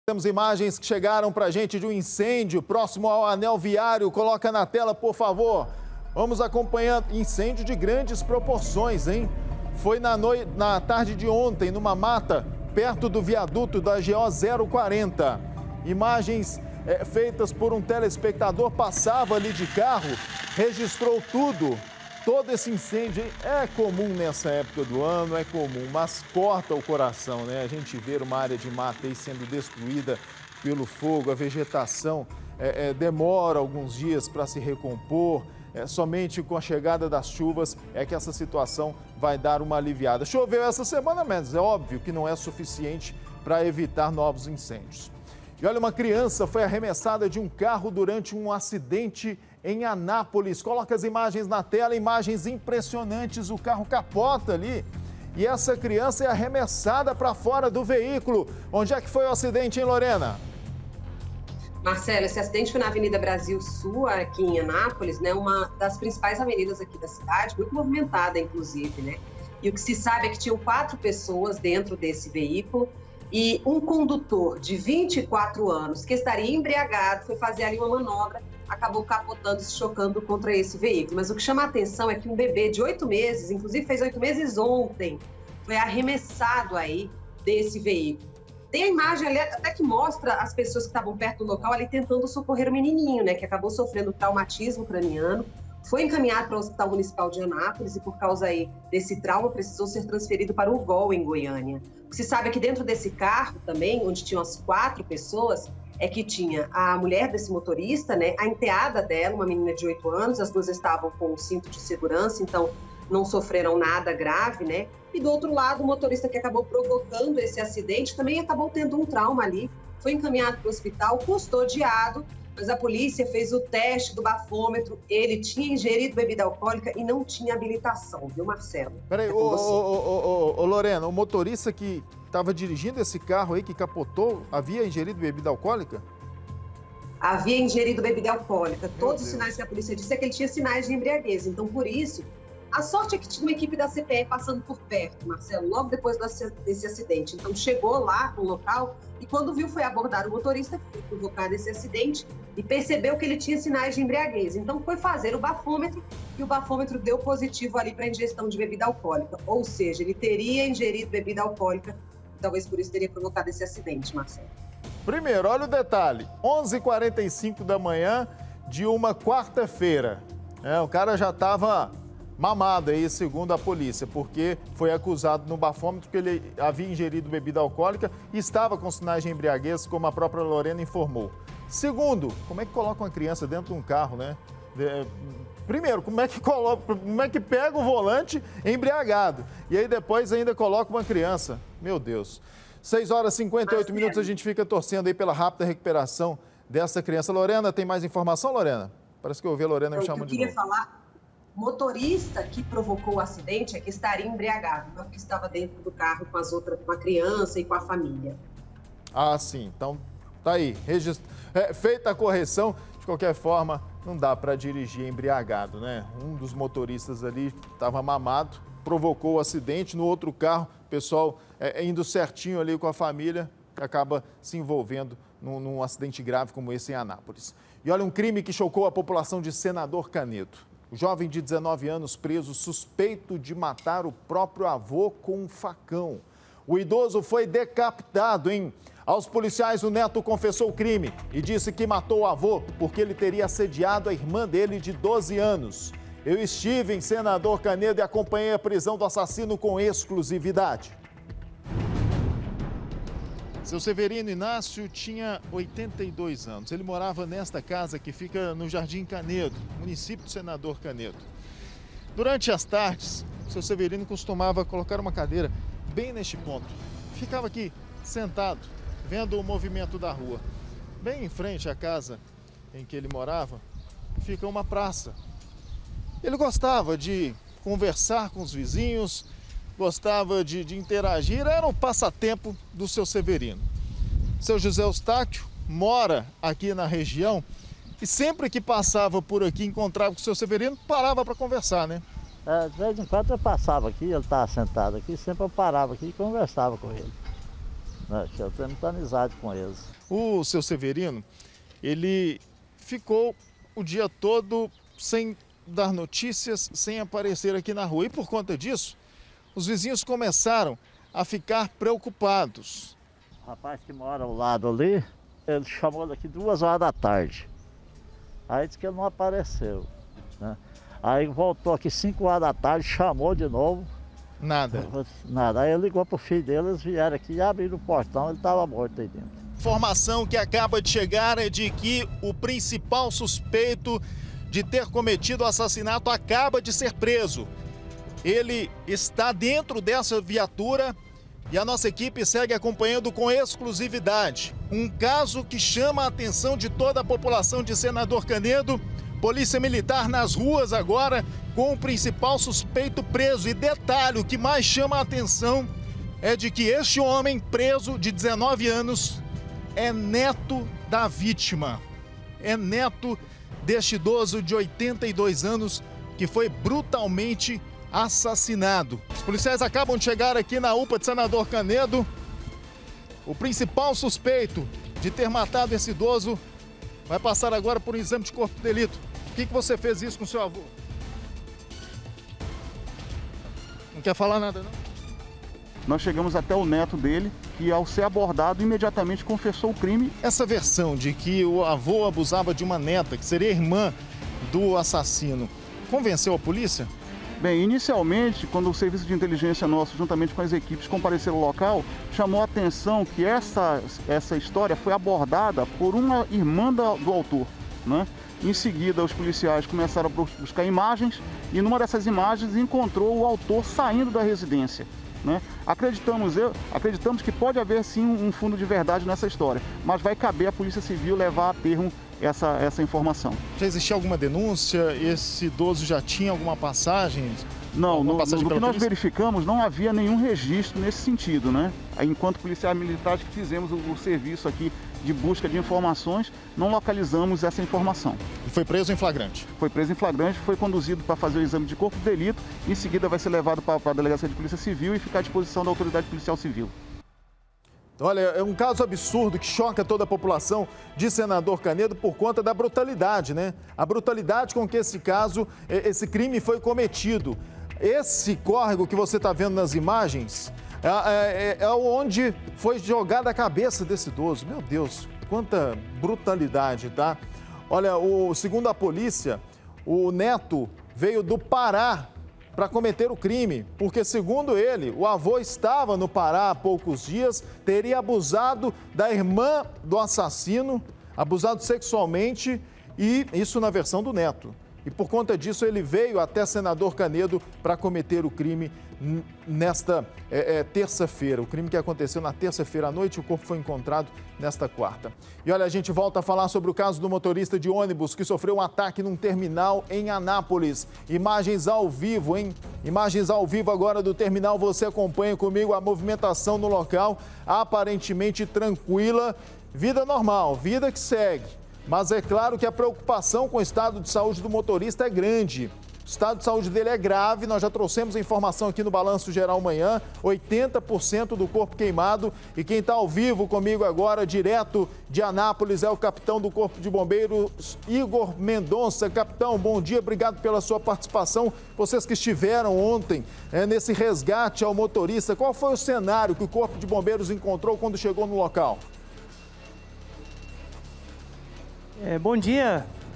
ATAQUE EM TERMINAL: MULHER ATEIA FOGO EM MOTORISTA DE ÔNIBUS
É GRAVE O ESTADO DE SAÚDE DO MOTORISTA DE ONIBUS QUE TEVE O CORPO INCENDIADO EM ANÁPOLIS. A MULHER QUE JOGOU ALCOOL NO CORPO DELE E ATEOU FOGO ESTÁ PRESA. VOU ATÉ A ANÁPOLIS COM A LORENA GOMES QUE TRAZ DETALHES DESSE CASO. O QUE JÁ FOI APURADO ATÉ AGORA?